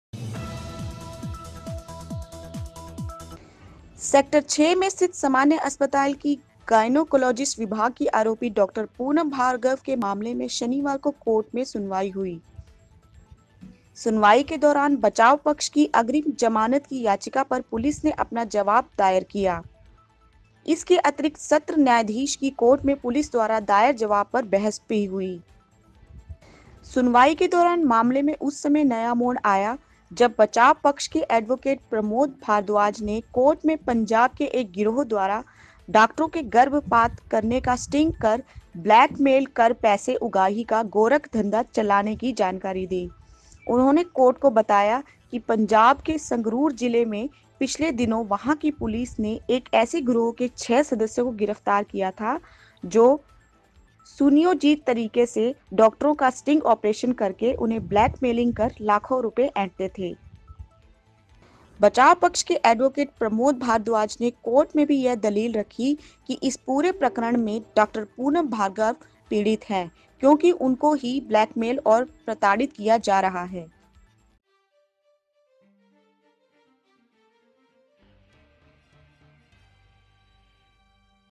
डॉक्टरों के स्टिंग करके ब्लैकमेलिंग का चल रहा गोरखधंधा
सुनवाई के दौरान मामले में उस समय नया मोड़ आ गया जब बचाव पक्ष के एडवोकेट प्रमोद भारद्वाज ने कोर्ट में पंजाब के एक गिरोह द्वारा डॉक्टरों के गर्भपात करने का स्टिंग कर ब्लैकमेल कर पैसे उगाही का गोरखधंधा चलाने की जानकारी दी। उन्होंने कोर्ट को बताया कि पंजाब के संगरूर जिले में पिछले दिनों वहां की पुलिस ने एक ऐसे गिरोह के छह सदस्यों को गिरफ्तार किया था, जो सुनियोजित तरीके से डॉक्टरों का स्टिंग ऑपरेशन करके उन्हें ब्लैकमेलिंग कर लाखों रुपये एेंठते थे।.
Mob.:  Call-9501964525